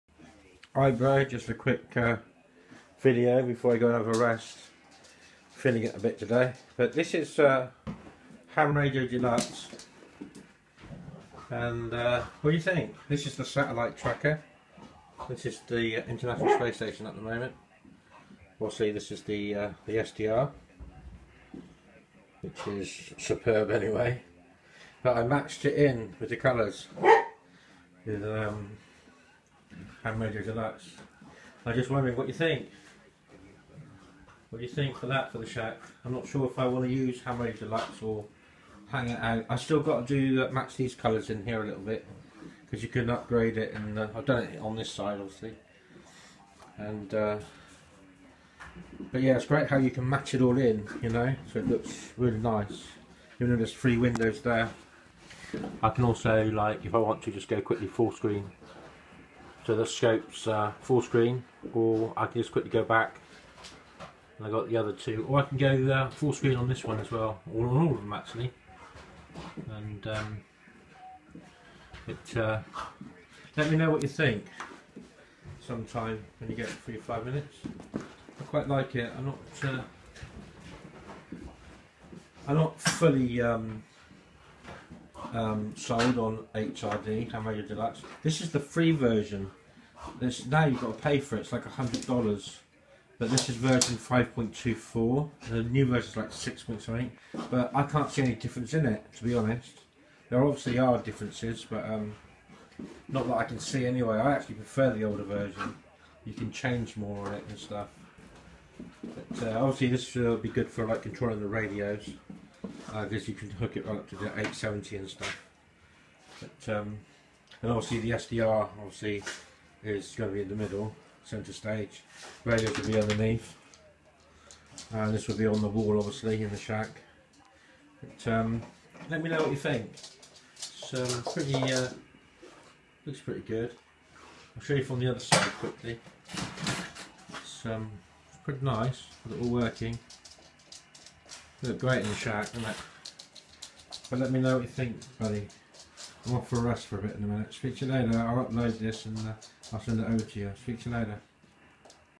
Quick video of Ham Radio Deluxe and SDR on super ultra wide Monitor
Ham radio deluxe 5.24 with Expert SDR Quick video to my Brother Phil 2E0SSB to see what he thinks of it on my new Ultra ultra wide monitor.